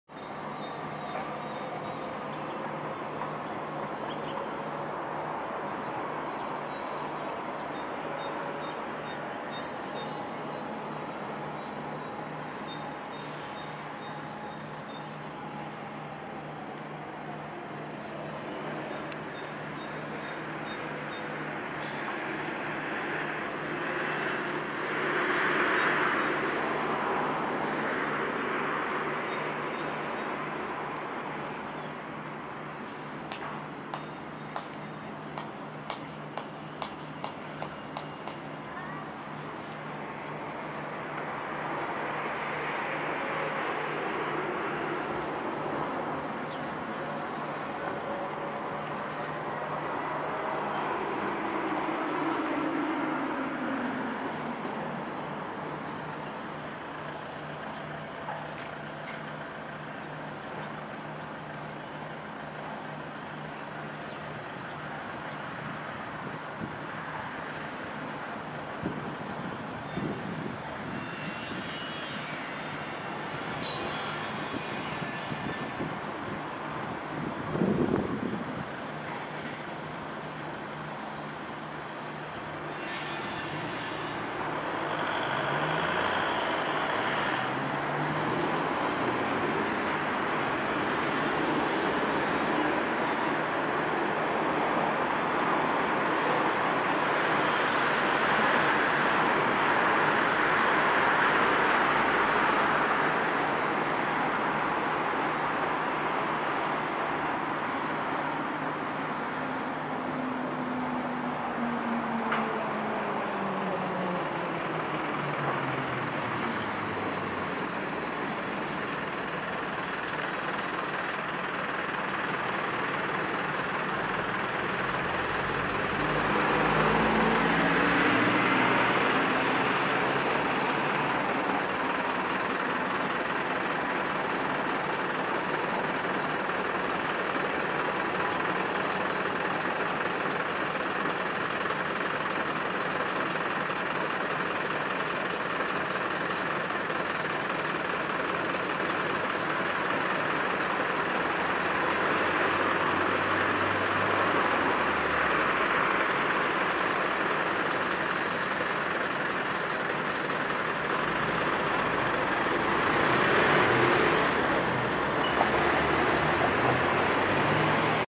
Swan in the trap
The next day he will be together with his swan wife again.